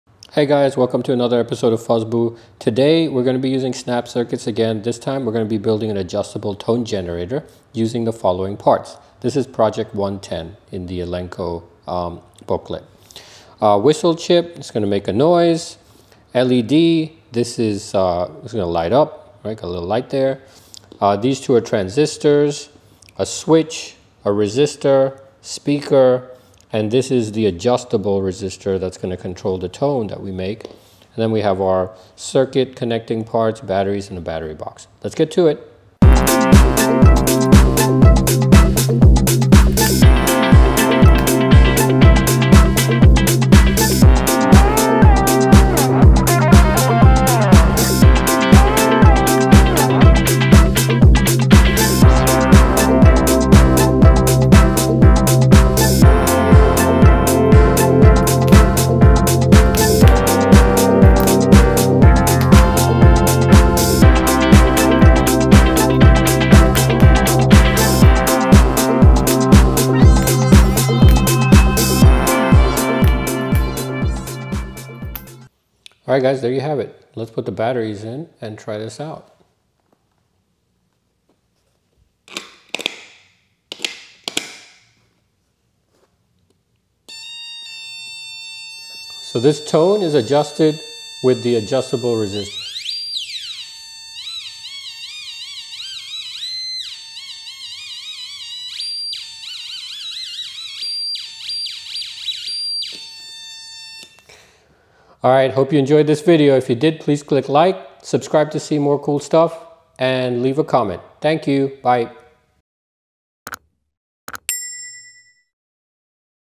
How to make a tone generator using Snap Circuits
Step by step build of a tone generator using Snap Circuits. Top down view allows you to see which parts go where to make it easy to follow along and build it yourself.

This tone generator has an adjustable resistor that allows you to control the tone pitch in real time to make some funky sounds.

Amazon links are provided below to products used in this video:

Snap Circuits Classic Kit